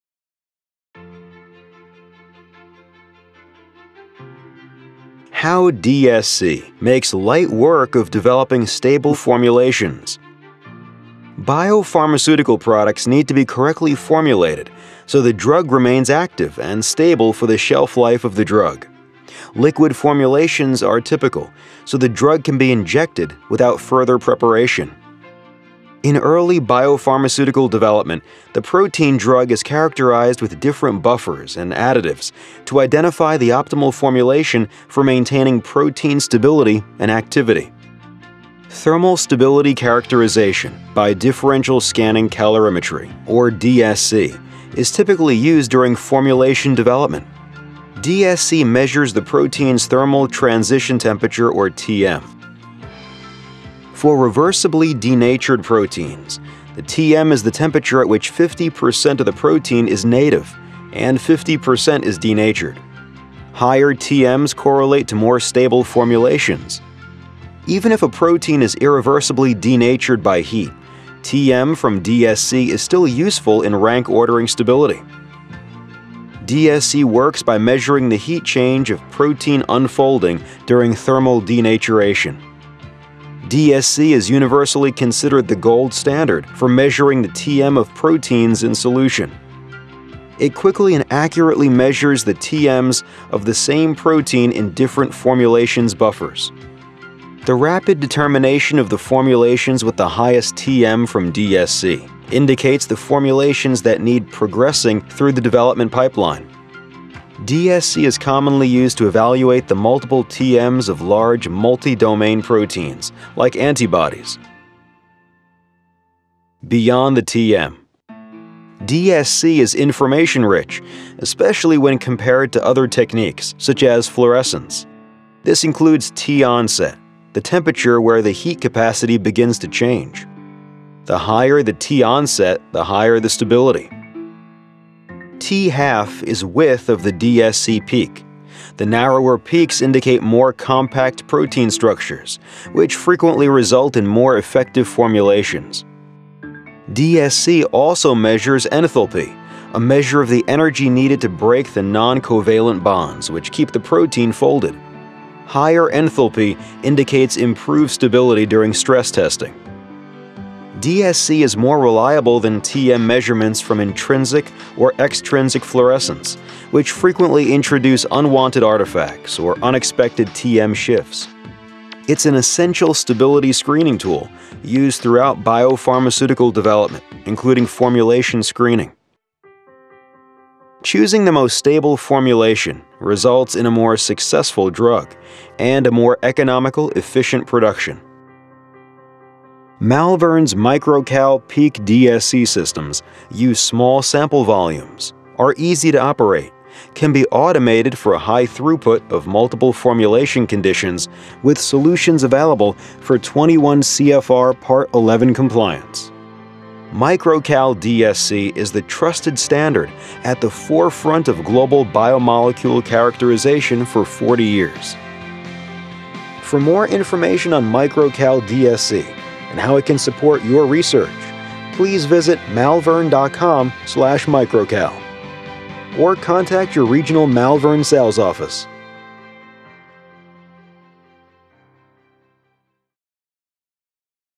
Malvern MicroCal DSC – Formulation stability testing
Biopharmaceutical Formulation Stability Testing – using Differential Scanning Calorimetry (DSC)

See how Differential Scanning Calorimetry (DSC) helps with biopharmaceutical formulation characterization from candidate molecule through to final drug product. Understand why MicroCal Differential Scanning Calorimetry (DSC) is the gold standard for thermal stability characterization of biotherapeutic formulations.  Ensuring good thermal stability is vital so that the protein remains safe and effective throughout the shelf life of the product.

Differential Scanning Calorimetry (DSC) helps measure the stability of proteins.